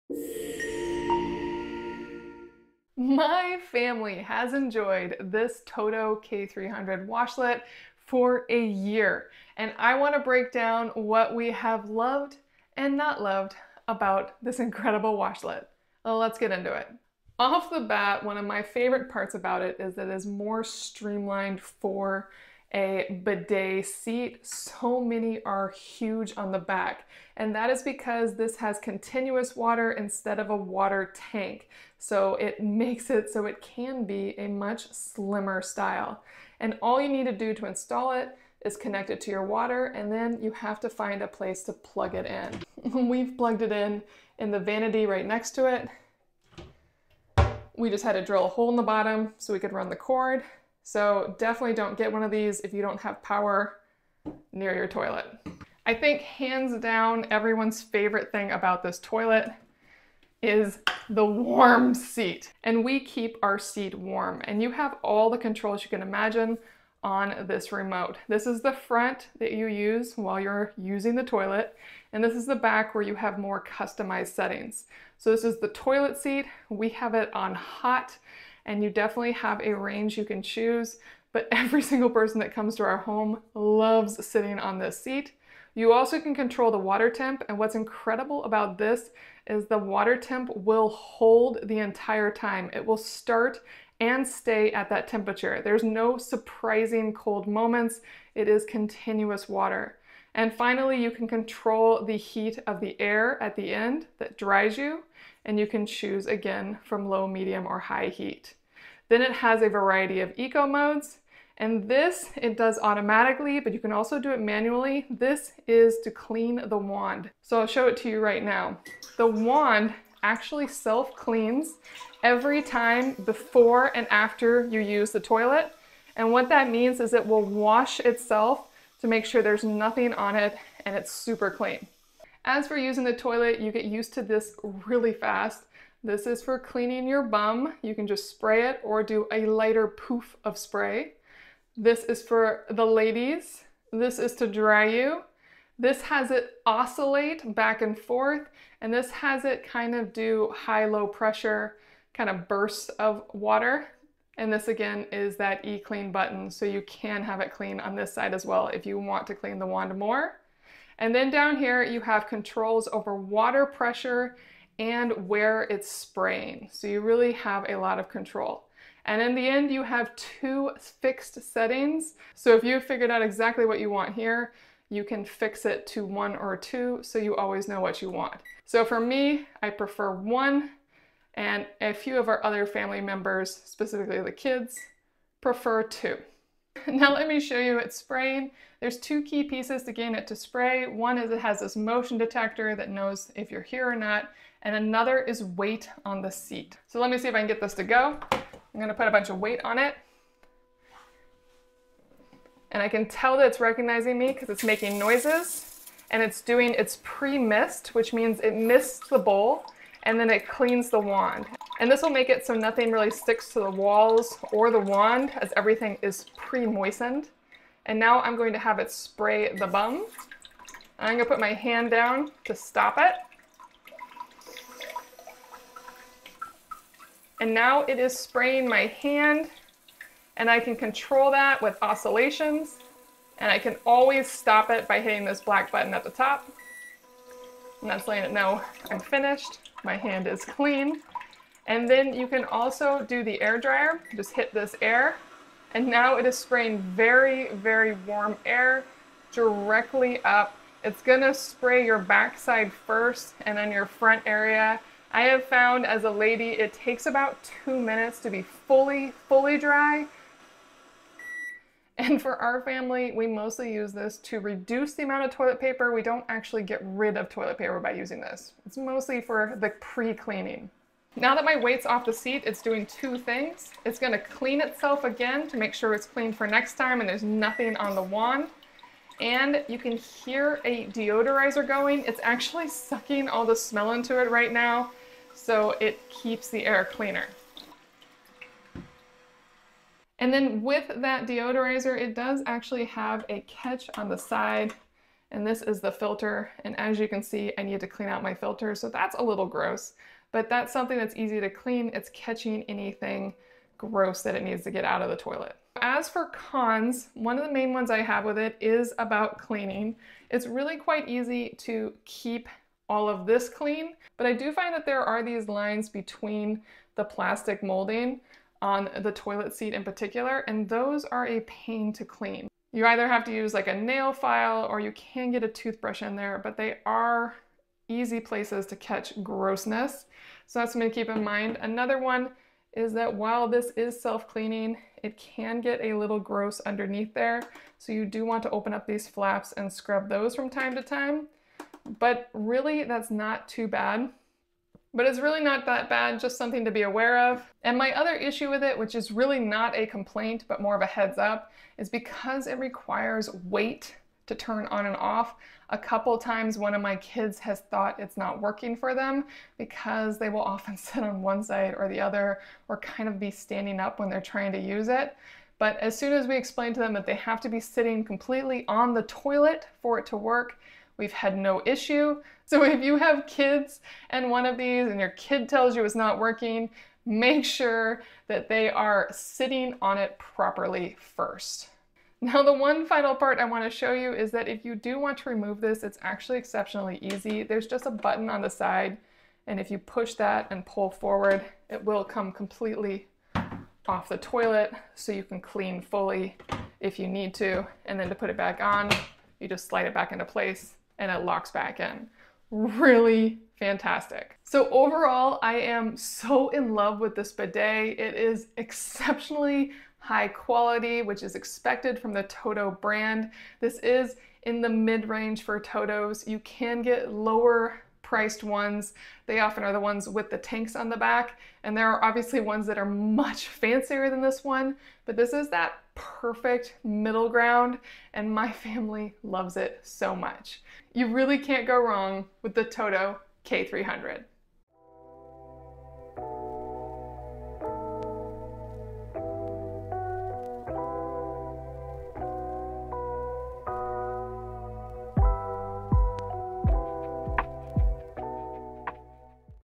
Loving Our Toto K300 Bidet After One Year - Full Breakdown
TOTO K300 Review: A Year of Luxury & Clean Comfort Experience (paid link)

Experience the ultimate luxury bathroom experience with our 1-Year Review of the Toto K300 Washlet Bidet Seat. Enjoy instant, continuous warm water, a heated bidet seat, and a customizable remote for personalized cleaning settings. Discover the benefits of the Air-In Wonder Wave, self-cleaning EWATER technology, and an air deodorizer that keeps your bathroom fresh. Perfect for families, the TOTO K300 offers seamless DIY installation and top-notch customer service. Elevate your hygiene and comfort with this premium bidet!